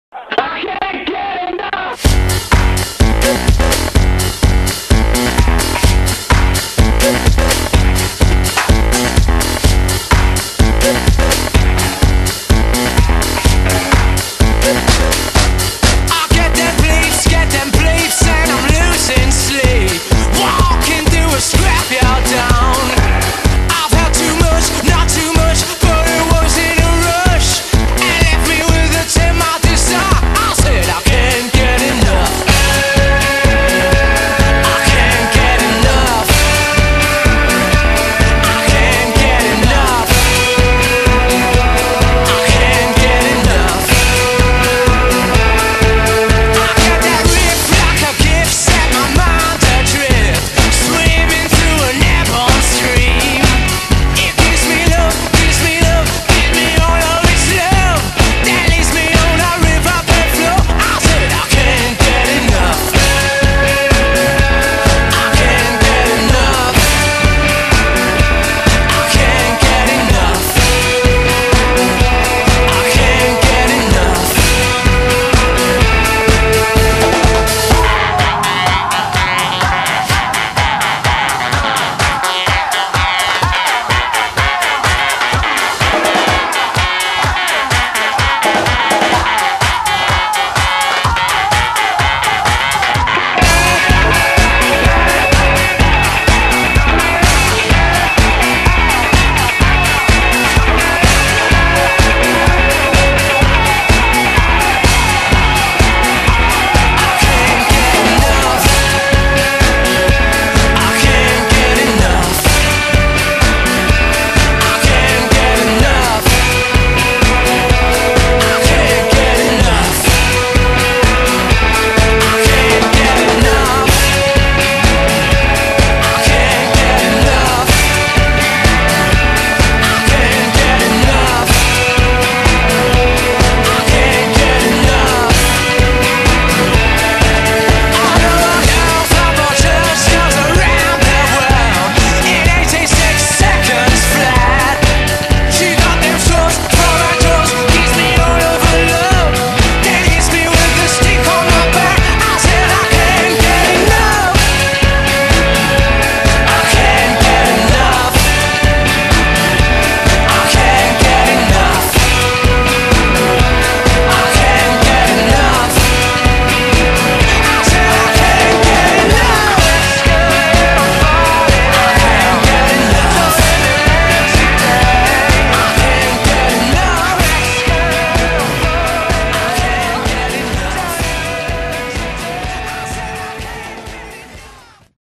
Telemark Skiing at the Stubaier Gletscher - 2009
Telemark skiing  - reedit
Bob en Mathijs gaan los